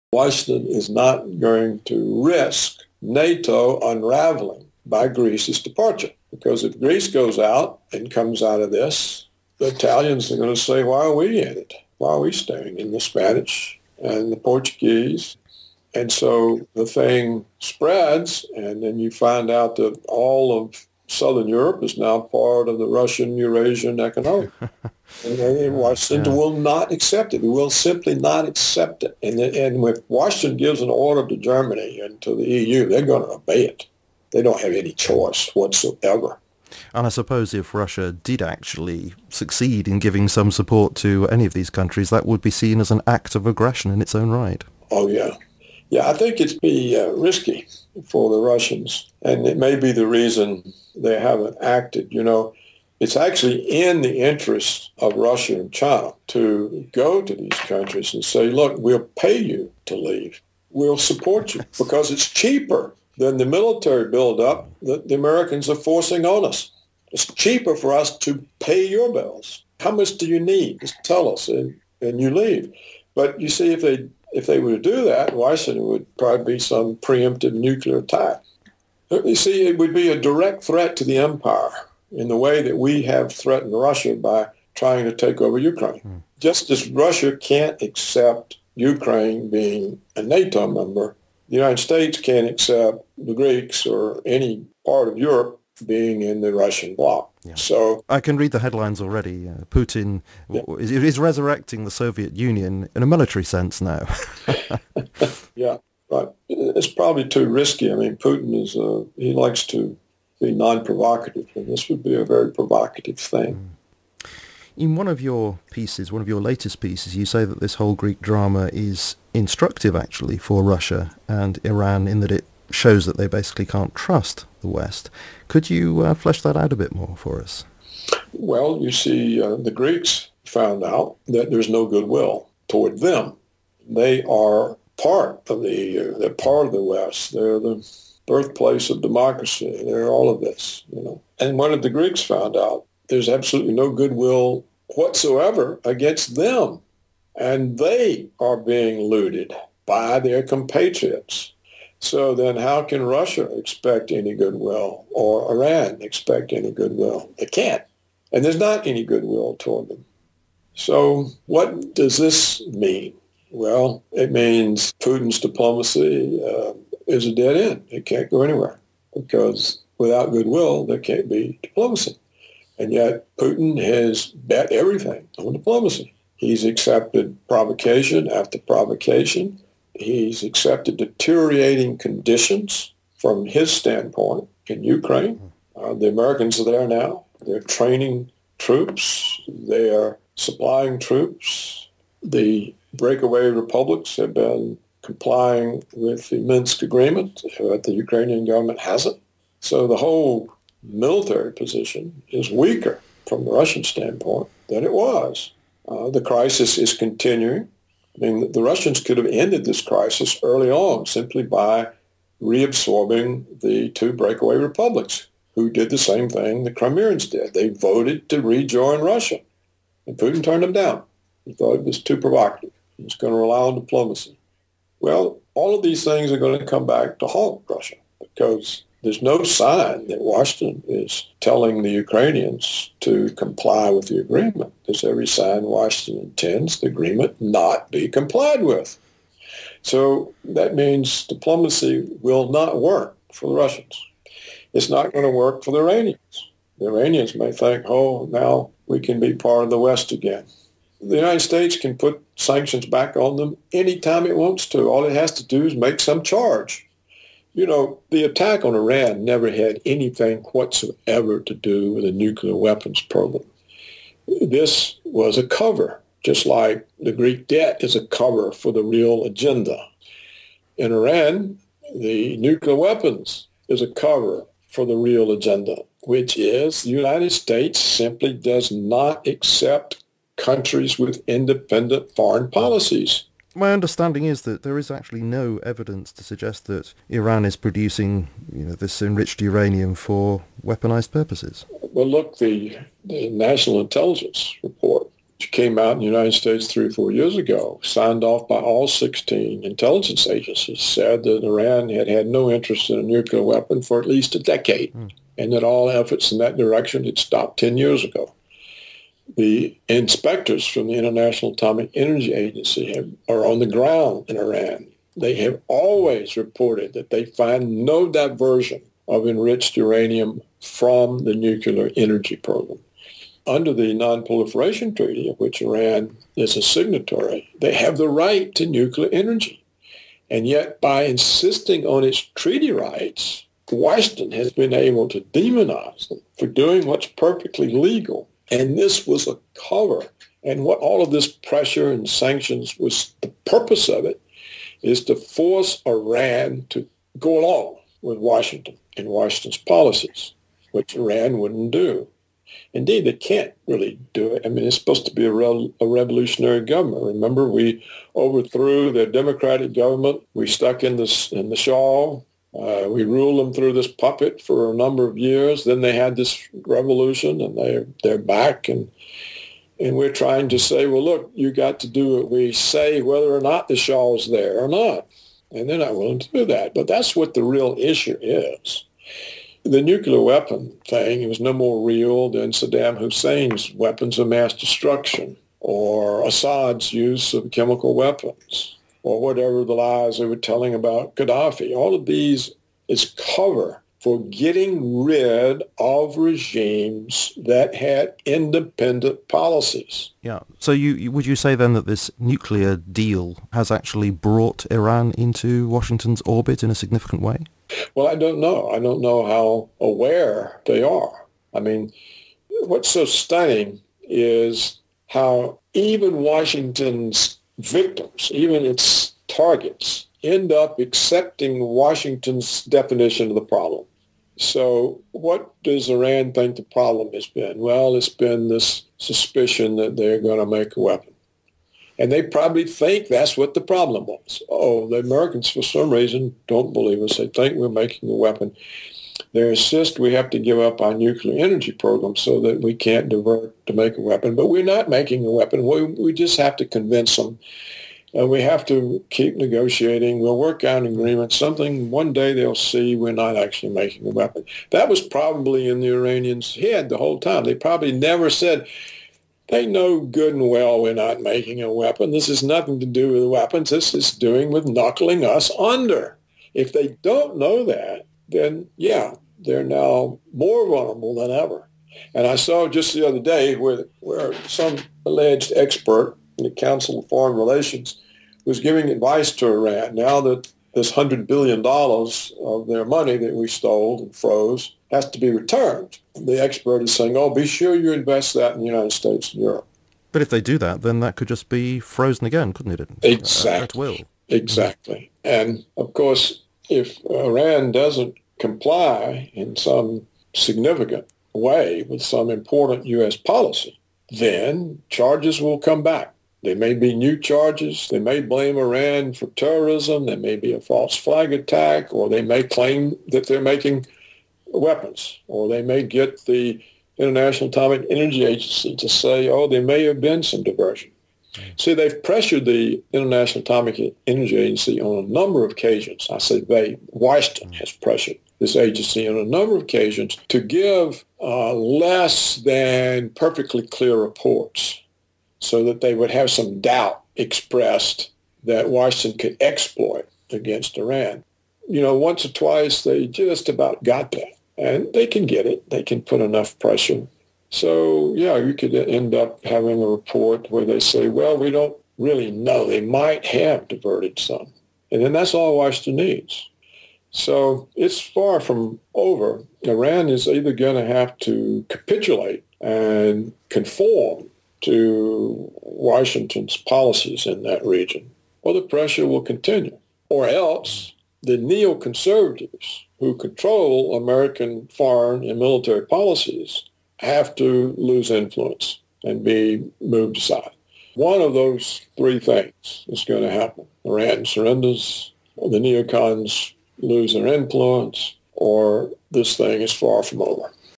Paul Craig Roberts on the nuclear program of Iran
Paul Craig Roberts (born April 3, 1939) is an American economist and a columnist for Creators Syndicate. He served as an Assistant Secretary of the Treasury in the Reagan Administration and was noted as a co-founder of Reaganomics. He is a former editor and columnist for the Wall Street Journal, Business Week, and Scripps Howard News Service. He has testified before congressional committees on 30 occasions on issues of economic policy.